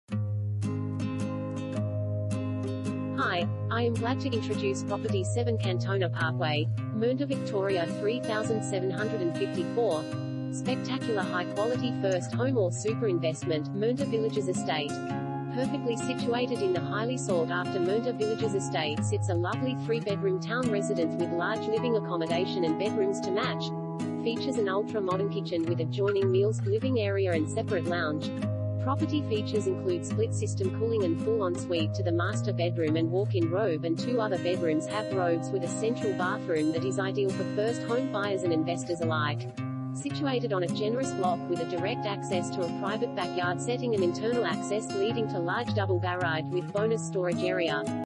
7 Cantona Parkway, Mernda VIC 3754 | AuPropertyReport.Com
7 Cantona Parkway, Mernda VIC 3754
Property Type: house
Bedrooms: 3
Bathrooms: 3
Car Space: 2
Spectacular High Quality First Home Or Super Investment - Mernda Villages Estate